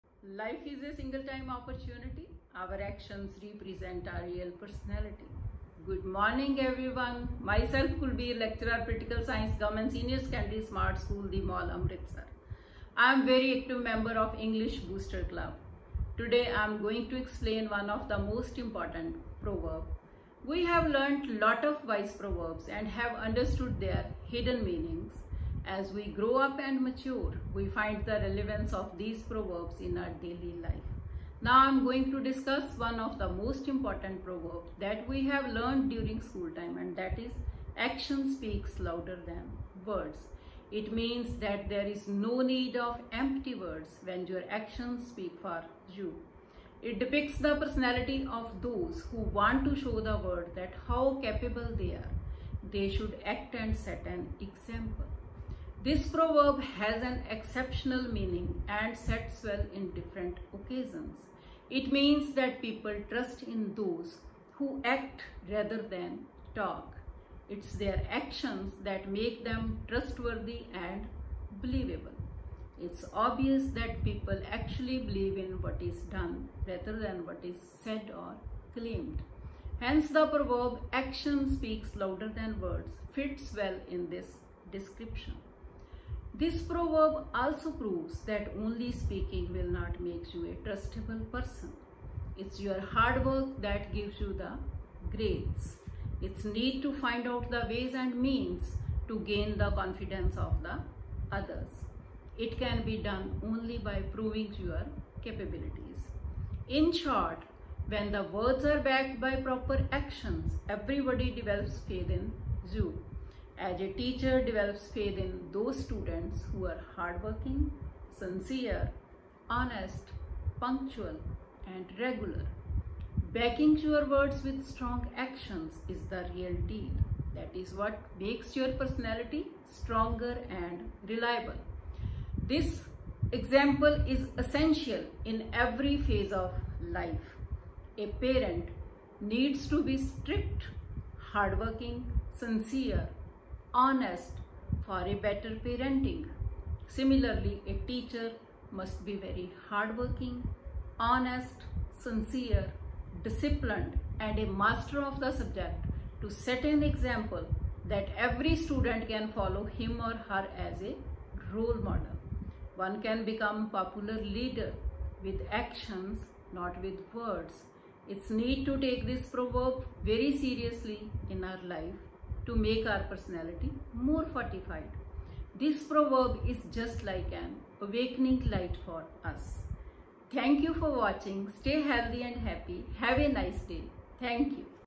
#EBC(English Booster Club activity)by Kulbir Lect.Pol.Science GSSSS The Mall Asr.
At Present scenario #EBC Is the Unique feature of Govt smart SChool of Punjab. Students and teachers get opportunity to participate Get confidence and relish their personalities.This-is platform where I encourage the students for English speaking and leadership qualities 🙏🙏🙏

EBC - Teachers
Name : Ms Kulbir Khiala
Lect. Politicoal science
GSSSS The Mall Asr.